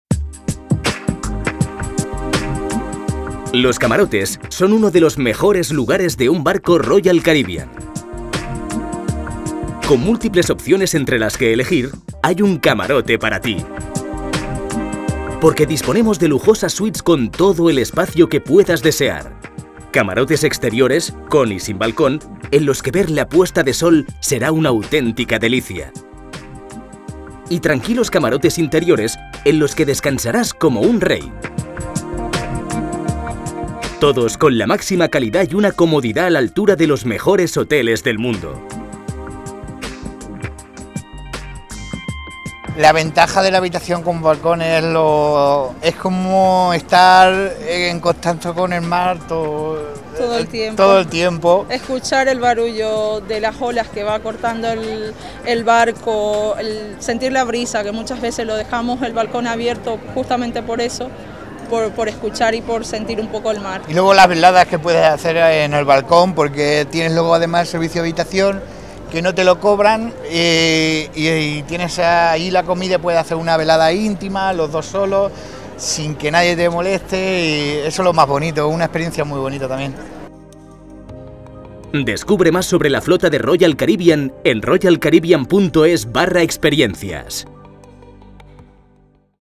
Catálogo 2013: Relájate y disfruta en los camarotes de Royal Caribbean
En cada barco existe muchas categorías de acomodación para que elijas la más adecuada para ti. Desde una suite a un camarote interior, encontrarás un mobiliario acogedor y un ambiente tranquilo donde descansar durante tus vacaciones. Y lo más cómo de todo, es que solo debes deshacer la maleta una vez.

Experimenta nuestra extraordinaria atención Gold Anchor en el servicio de habitaciones las 24 horas del día.